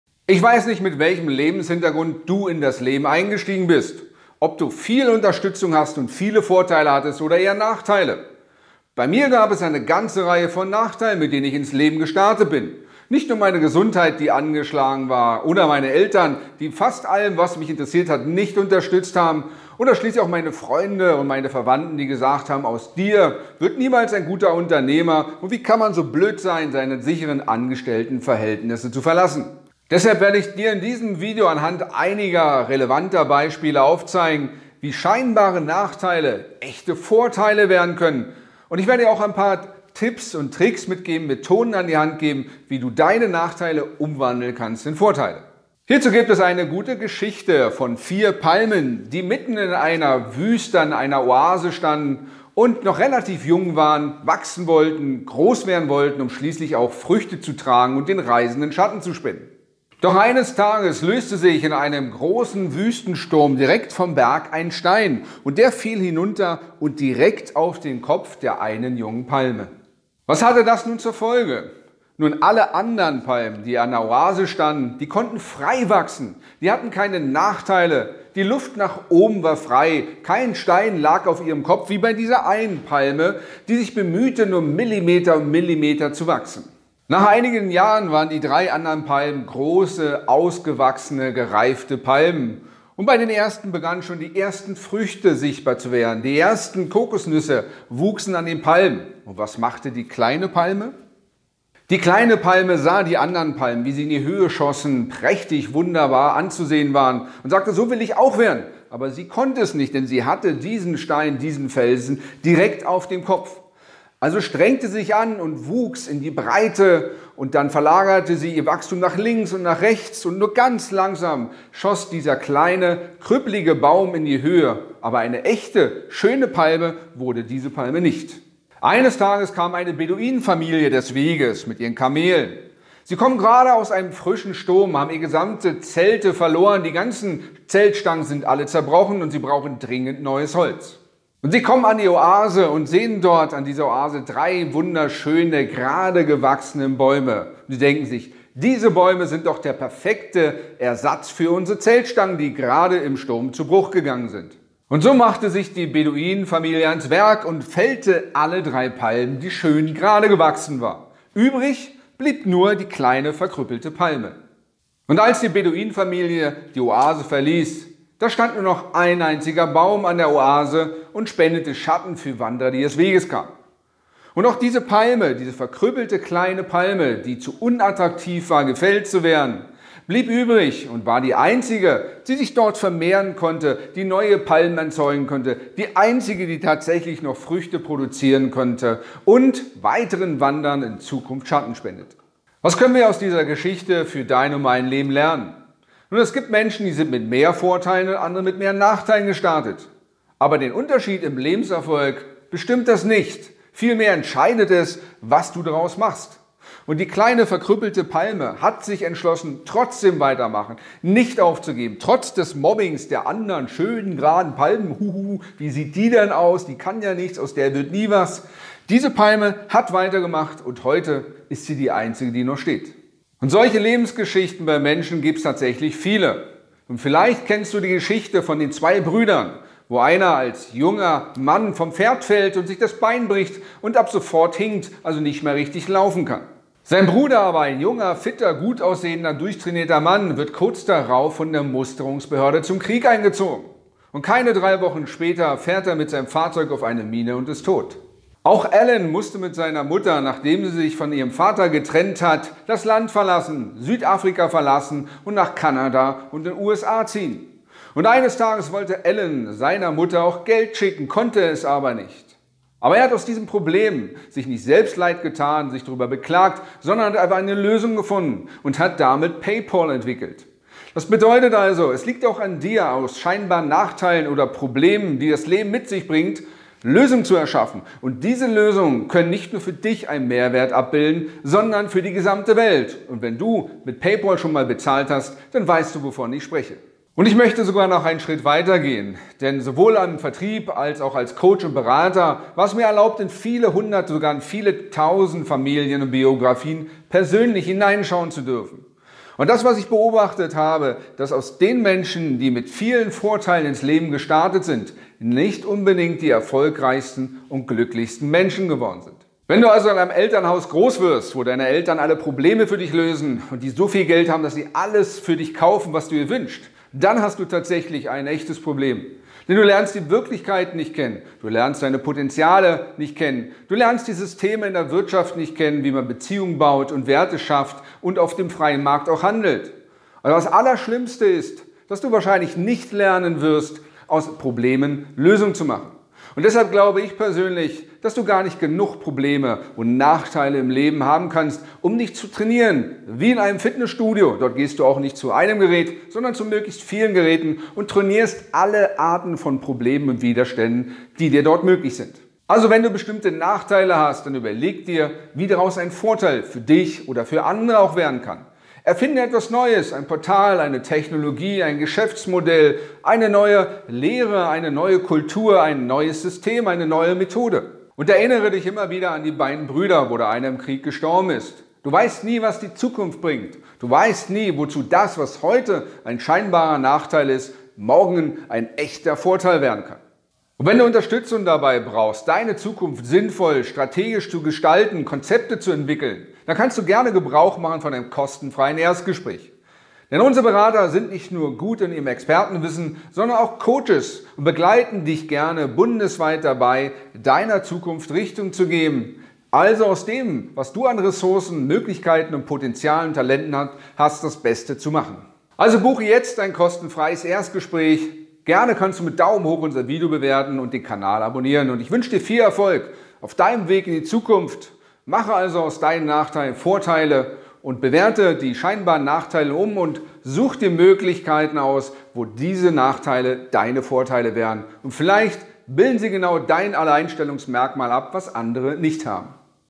Warum Nachteile Vorteile sein können
Wie gehst du mit Nachteilen und schlechten Erlebnissen um? Siehst du darin schon deinen Vorteil? Wenn nicht, dann lass dich von Daniel Schäfers Geschichte ermutigen.